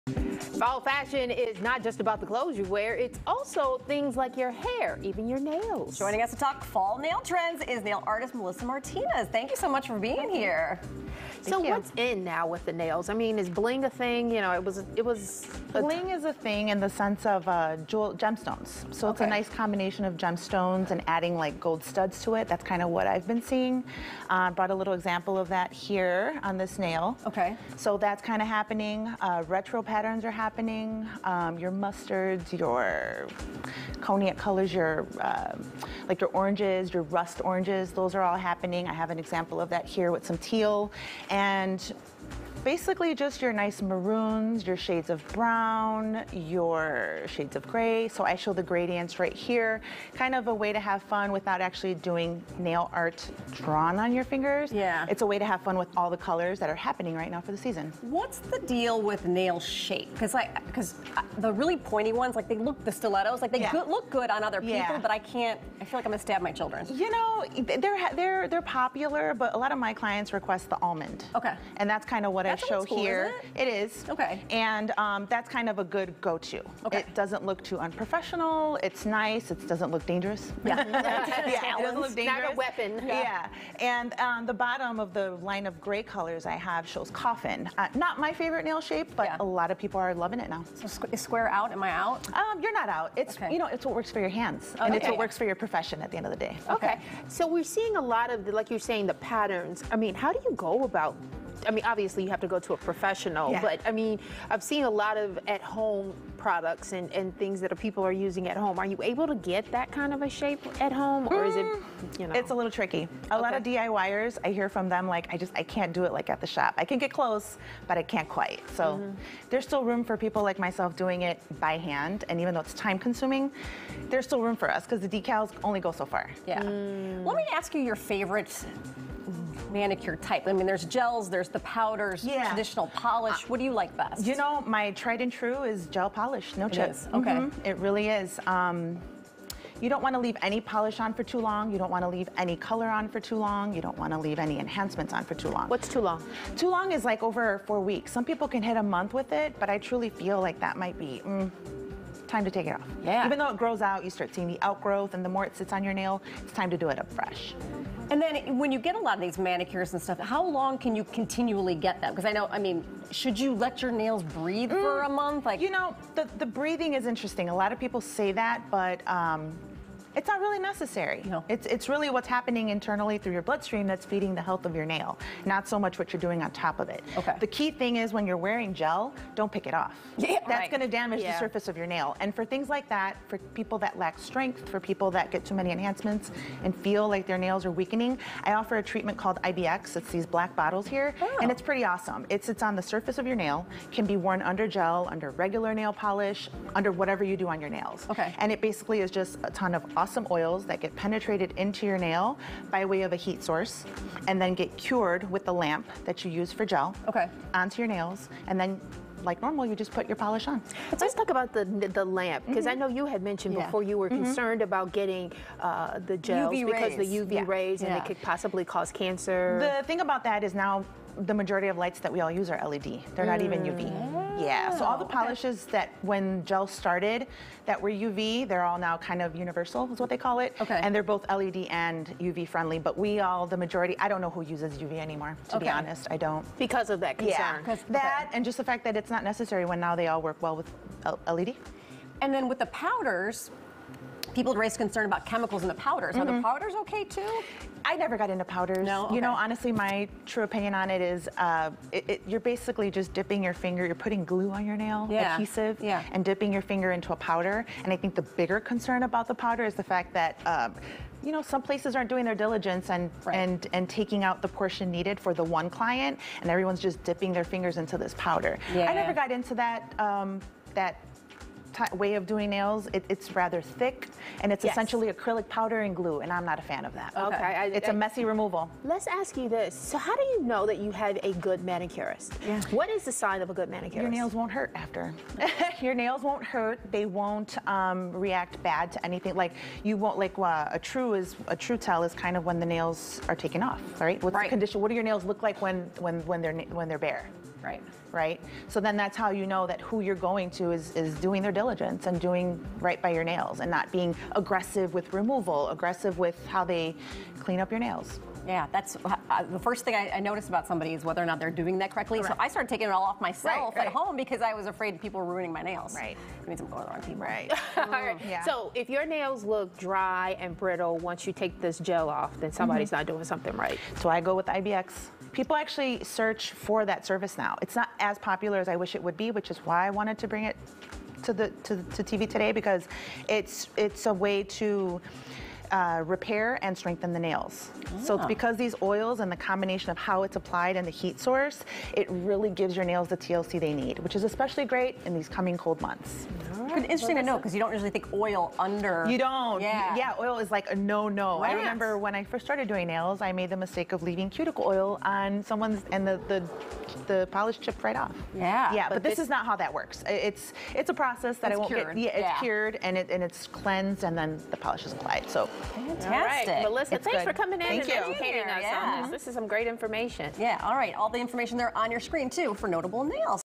Fall nail art trends: Color, design and care
Fall fashion is not just about the clothes you wear, it also includes things like your hair, even your nails.